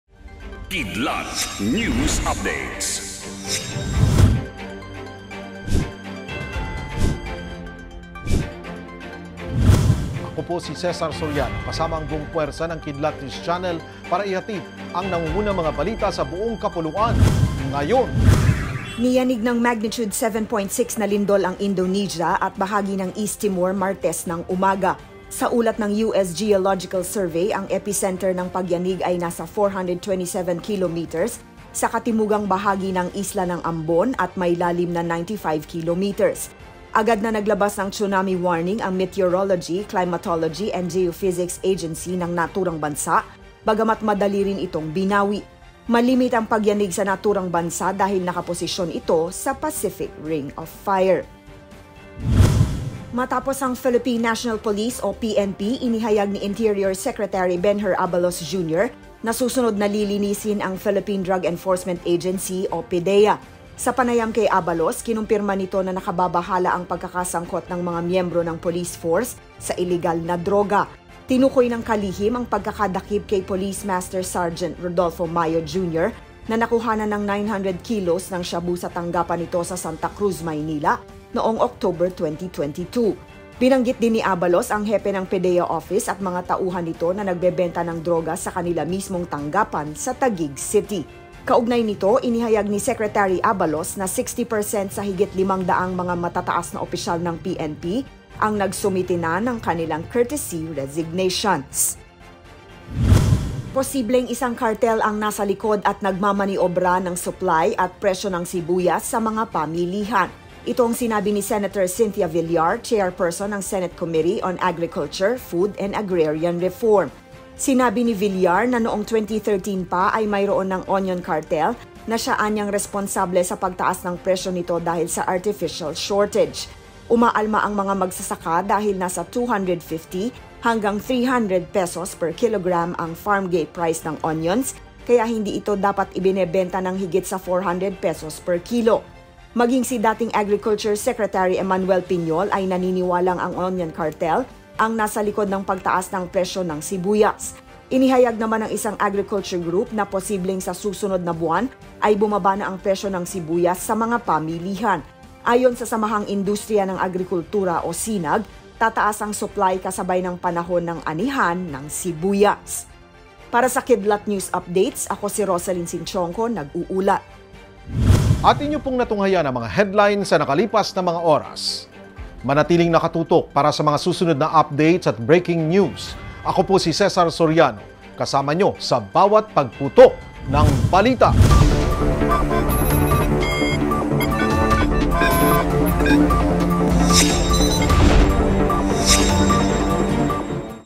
Internal Cleansing sa Police Force | Kidlat News Update (January 10, 2023 10AM)
Matapos ang Philippine National Police, inihayag ni Interior Secretary Benhur Abalos, Jr. Na susunod na lilinisin ang Philippine Drug Enforcement Agency o PDEA. Sa panayam kay Abalos, kinumpirma nito na nakababahala ang pagkakasangkot ng mga miyembro ng police force sa iligal na droga. Tinukoy ng kalihim ang pagkakadakip kay Police Master Sgt. Rodolfo Mayo, Jr. na nakuhanan ng 900/kg ng shabu sa tanggapan nito sa Sta. Cruz, Maynila noong October 2022. Binanggit din ni Abalos ang hepe ng PDEA Office at mga tauhan nito na nagbebenta ng droga sa kanila mismong tanggapan sa Taguig City.  Kaugnay nito, inihayag ni Secretary Abalos na 60% sa higit limang daang mataas na opisyal ng PNP ang nagsumite na ng kanilang courtesy resignations.

Ngayon Na!